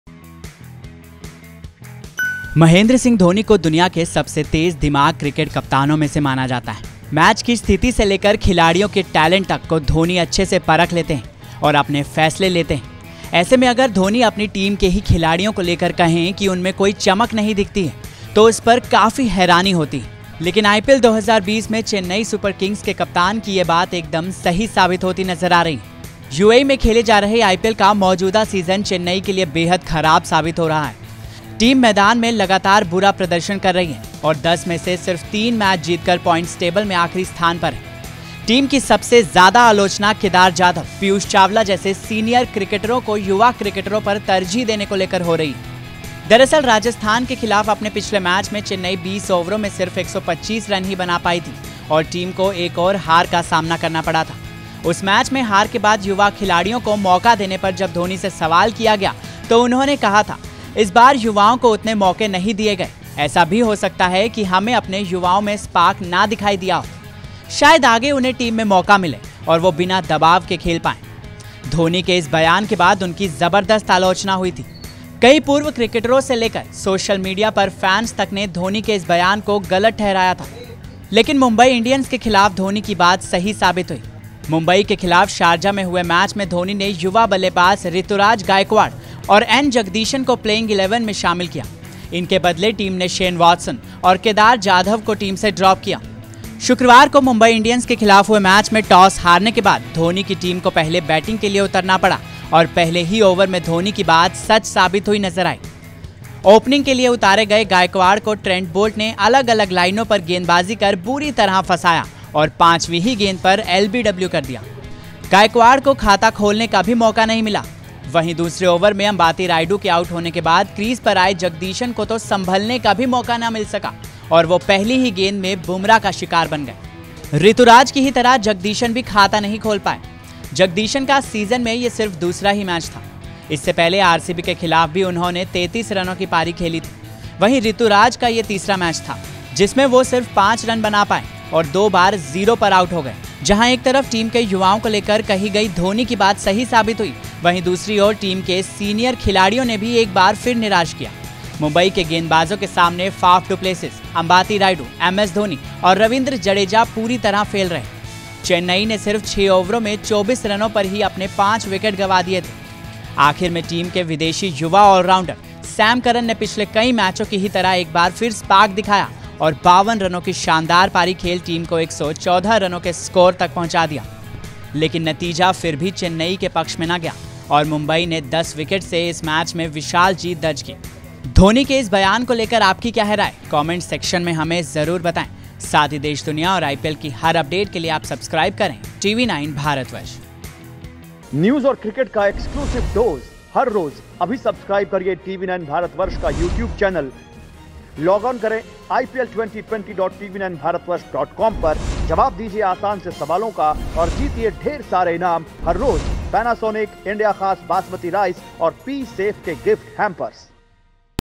CSK के युवा खिलाड़ियों को लेकर सच हुई Dhoni की बात, MI के खिलाफ एक के बाद एक चलते बने बल्लेबाज
महेंद्र सिंह धोनी (MS Dhoni) को दुनिया के सबसे तेज दिमाग क्रिकेट कप्तानों में से माना जाता है. मैच की स्थिति से लेकर खिलाड़ियों के टैलेंट तक को धोनी अच्छे से परख लेते हैं और अपने फैसले लेते रहे हैं. ऐसे में अगर धोनी अपनी टीम के ही खिलाड़ियों को लेकर कहें कि उनमें कोई चमक नहीं दिखती है, तो उस पर हैरानी जरूर होगी, लेकिन IPL 2020 में चेन्नई सुपर किंग्स (Chennai Super Kings) के कप्तान की ये बात एकदम सही साबित होती दिख रही है. UAE में खेले जा रहे IPL का मौजूदा सीजन चेन्नई के लिए बेहद खराब साबित हुआ है